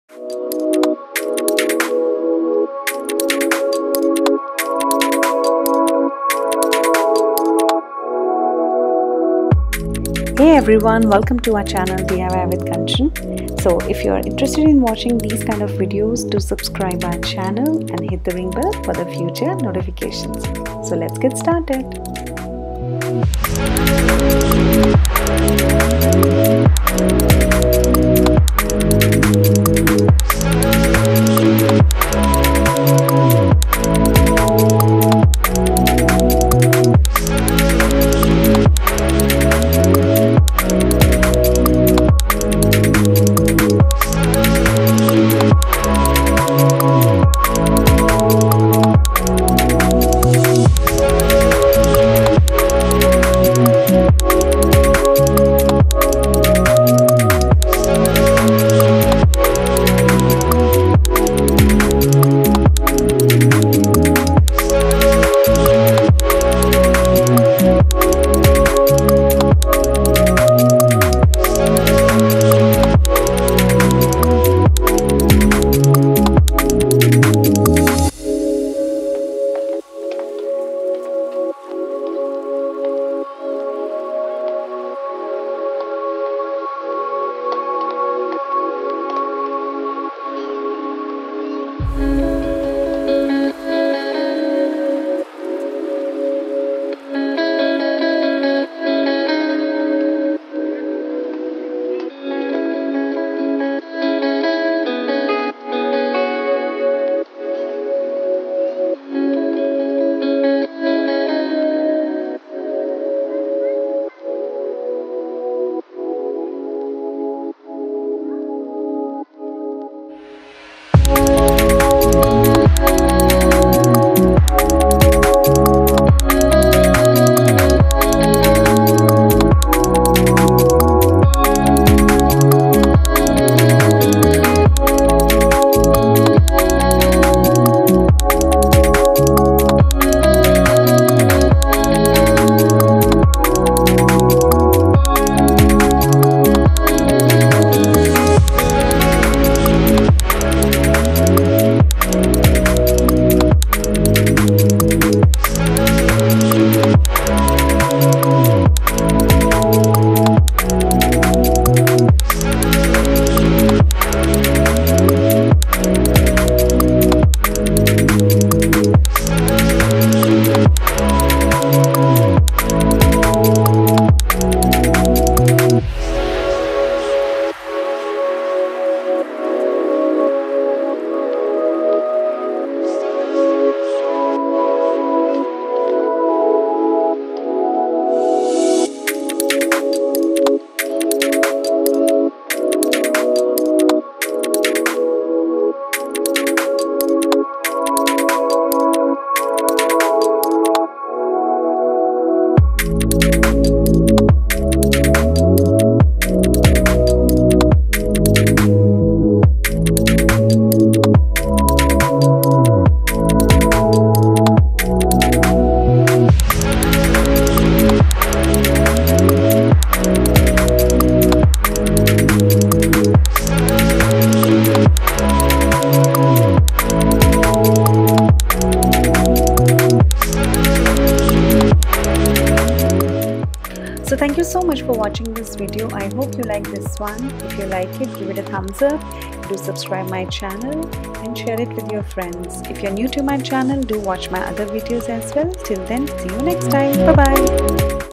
Upcycle Your Old Clutch/Purse/Bag|HACK for OLD PURSES - Easy REVAMP|Decoupage On Leather Bag/Purse
Hello Everyone, In this video I will be taking my old purse and I will convert them into this absolutely new and refresh purse.

👉Things you need to make this project :

1. Purse/ Bag
2. Decoupage napkin -
3. Modpodge glue
4. Paint brush
5. Varnish

How to make modpodge glue :

Take 500 grams Favicol glue and add 250 grams of water, mix it well and your decoupage glue is ready.

Love

DIYwithKANCHAN